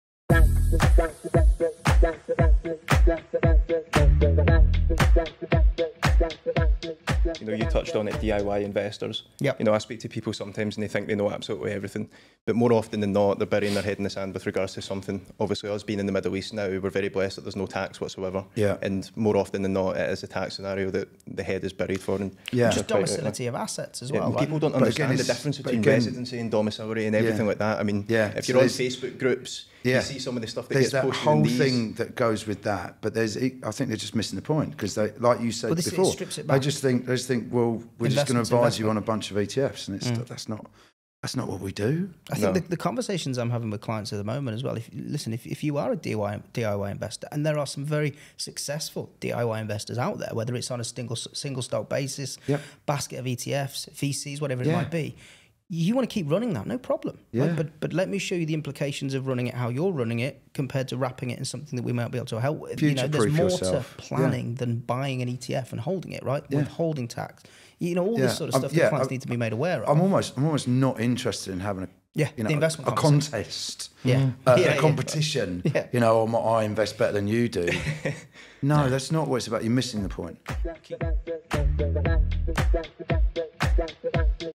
The Dangers Of DIY Investing For Expats
Skybound Wealth Management Advisers -  Jonathan Lumb, David Neville, Paul Waterman and Paul Butler discuss the dangers of DIY investing for expats.

For many investors in the Middle East, common misunderstandings tend to centre around tax requirements, domicile of assets, and the value that a qualified financial adviser can bring to your investment strategy.